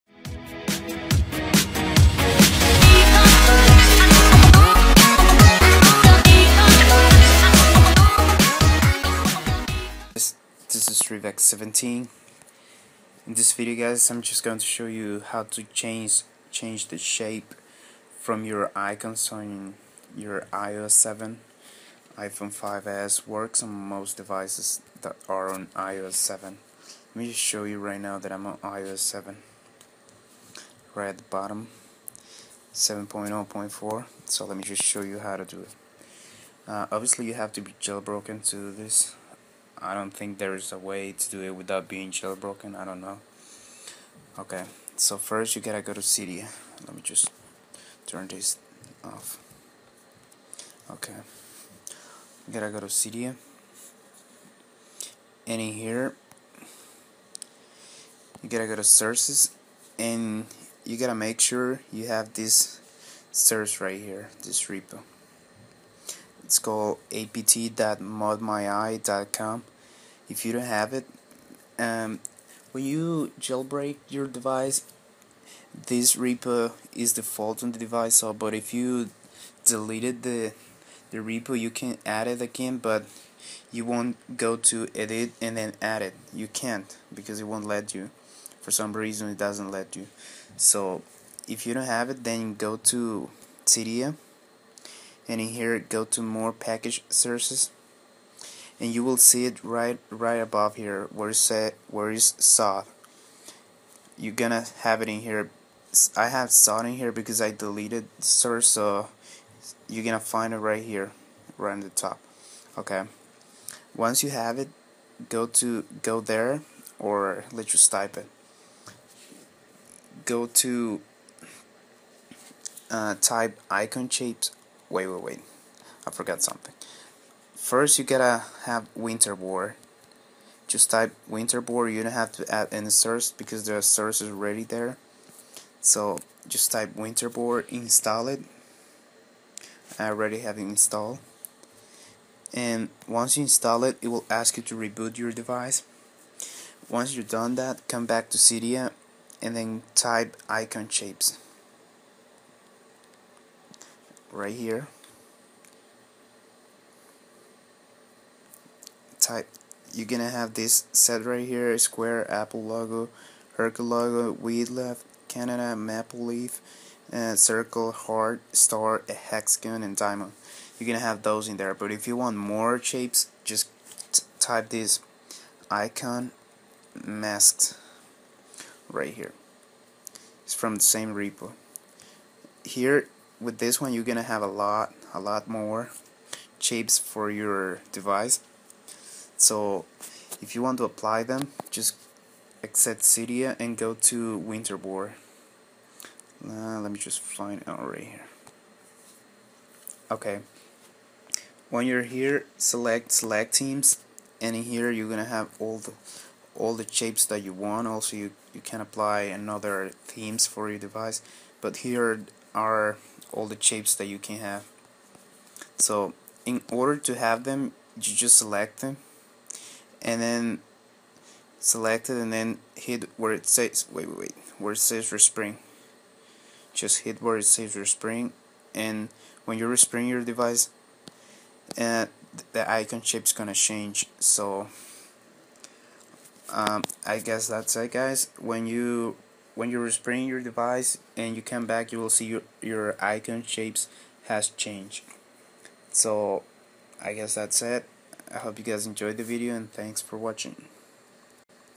How to Change Your Home Screen Icon Shapes on iPhone/iPad iOS7
this will change the shape of your icons to any desired shape u want

If you can't find winterboard this is the repo: Apt.thebigboss.org/repofiles/cydia/